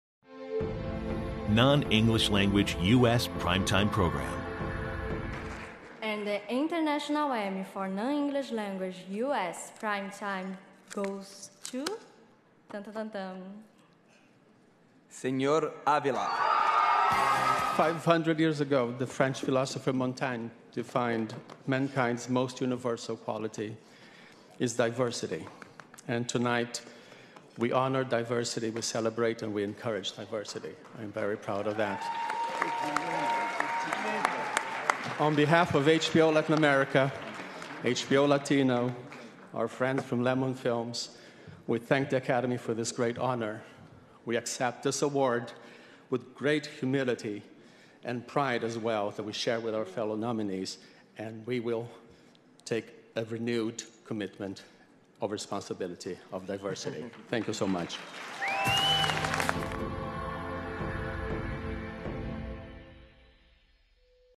"Sr. Ávila" gana el Emmy 2017
2017 Non-English Language US Primetime Program Winner, Sr. Ávila (USA) presented by Marina Ruy Barbosa and Renato Góes at the 45th International Emmy® Awards, November 20, 2017 in New York City

La serie producida por @thelemonstudios que cuenta con la participación de Tony Dalton SrÁvila se llevó el Emmy por la categoría a Mejor Programa en Horario de Mayor Audiencia en Lengua No Inglesa ¡Muchas felicidades!

FOLLOW US 👍🏻